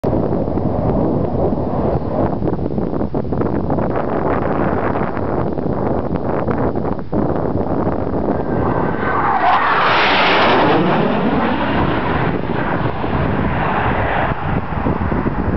Blue Angels Practice Fort Worth Texas May 11 2006
Checking out the area and getting it right on a perfect day for flying.